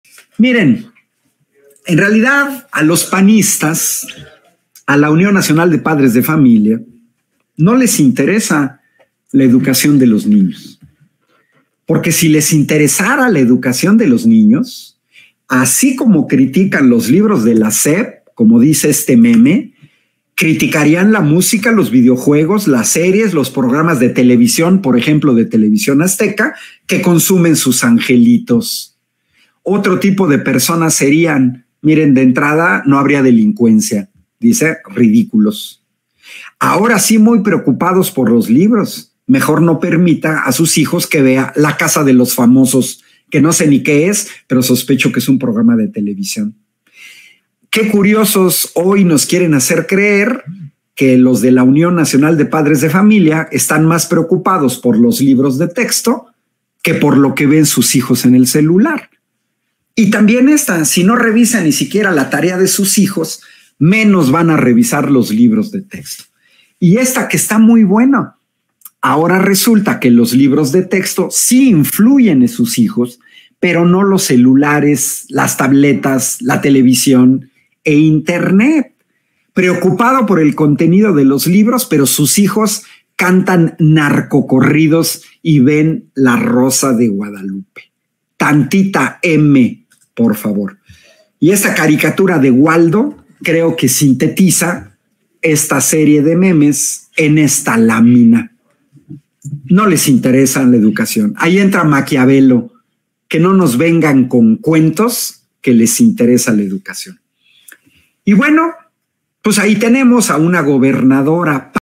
PANISTAS, ASÍ COMO CRITICAN LOS LIBROS DE TEXTO, QUE LO HAGAN CON CONTENIDO DE TV AZTECA
El doctor Ángel Balderas Puga nos comparte que a los panistas no les interesa la educación de los niños, así como critican los libros de la SEP, deberían criticar la música y contenidos en televisión.